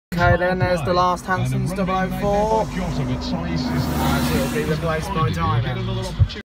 Last Hansons 004
The 004 Runs From Merry Hill To Harborne Birmingham Vis Halesowen